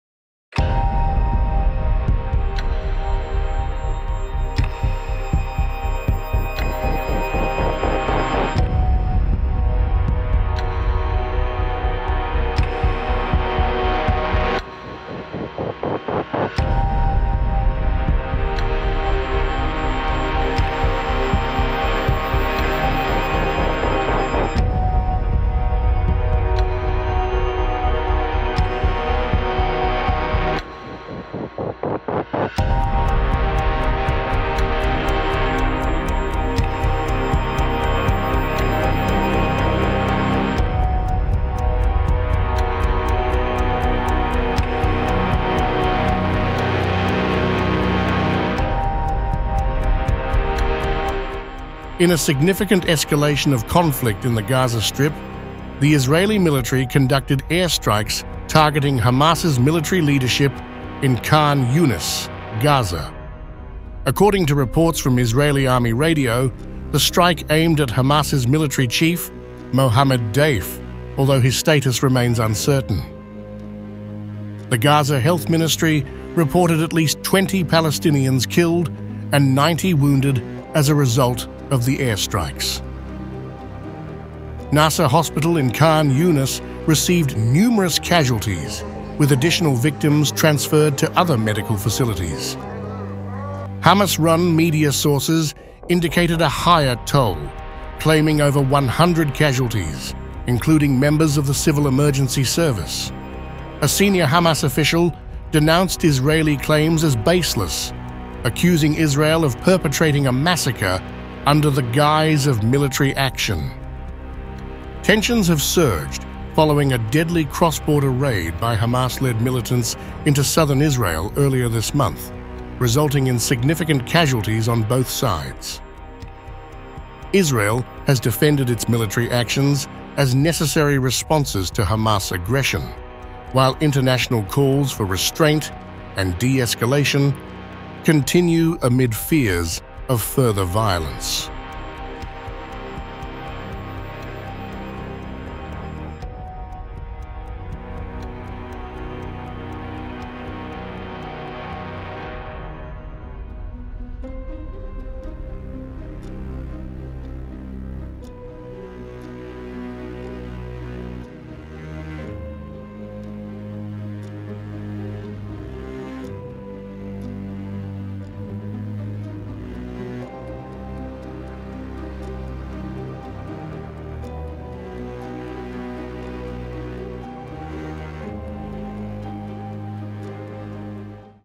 Israeli Strike in Gaza Targets Hamas Military Chief Amid Escalating Violence | Times Now World
Israeli military airstrikes in Khan Younis, Gaza, have targeted Hamas' military leadership, including the reported focus on Mohammed Deif, Hamas' military chief. The strikes have resulted in a significant toll, with conflicting casualty reports between Israeli and Hamas sources amidst heightened tensions in the region. The incident follows a recent Hamas-led raid into southern Israel, prompting international appeals for restraint amid fears of escalating conflict.